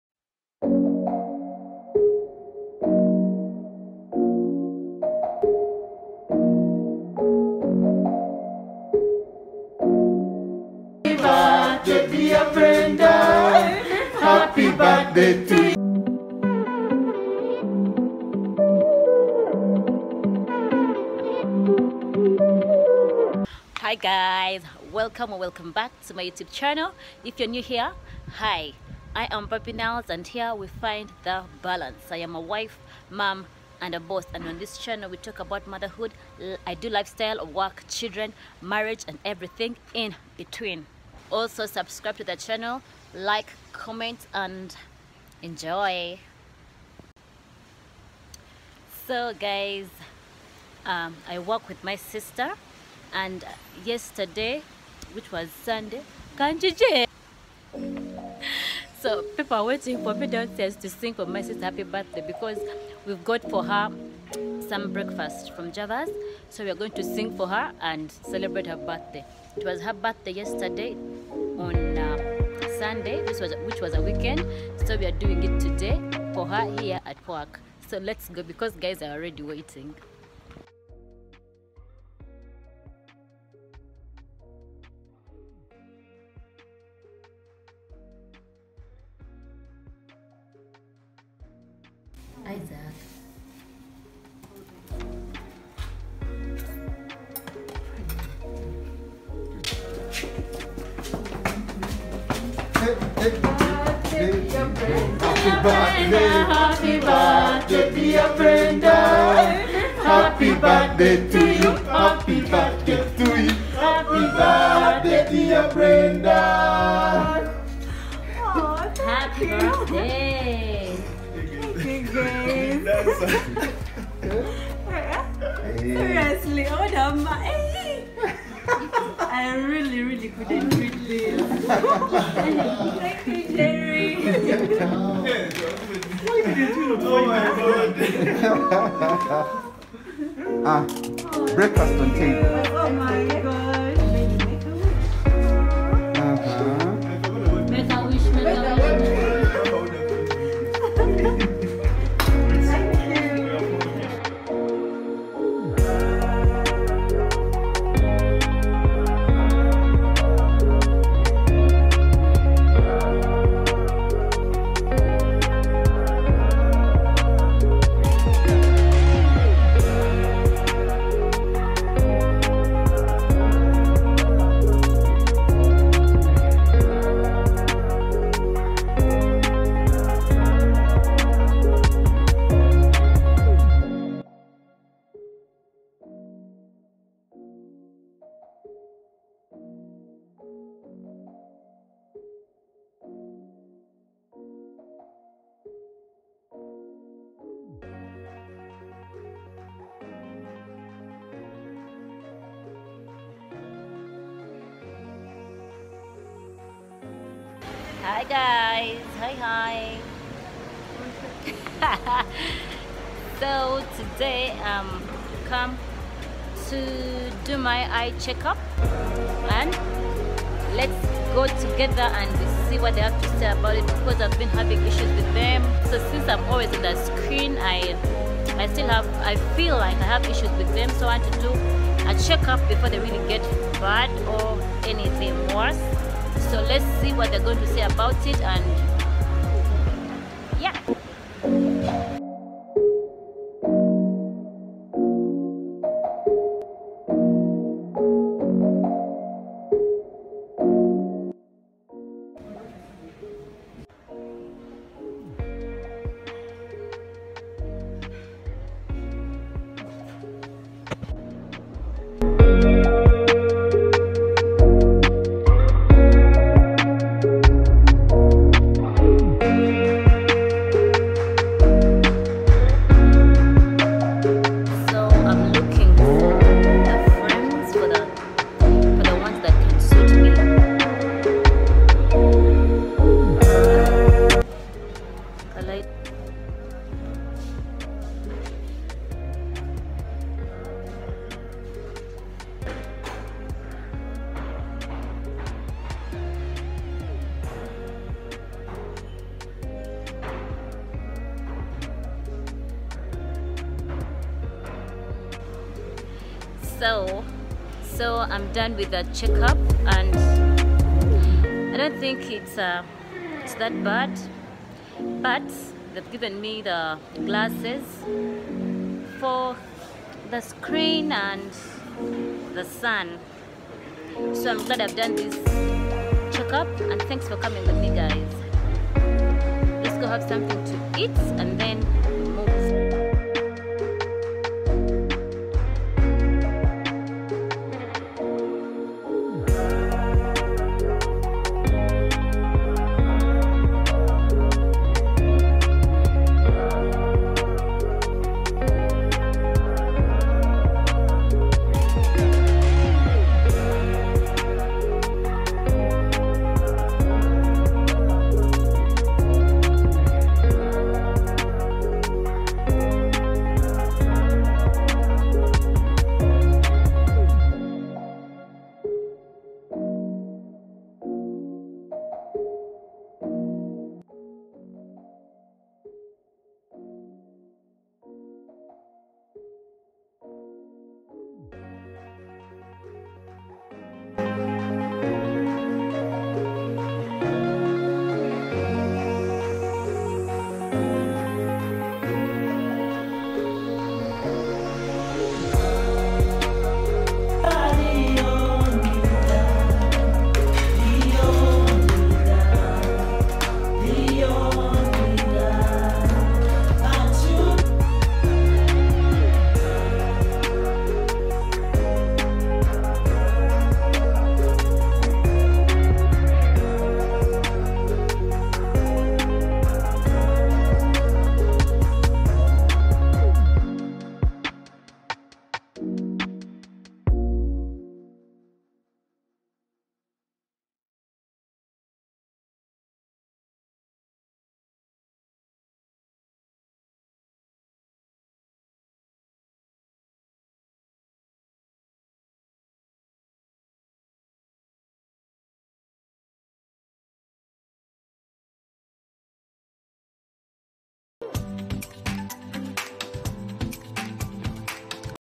WEEKLY VLOG://Birthday at Work/Eye Checkup/Baby Shower/ Ugandan YouTuber
Hey guys in this video am sharing how my week was like.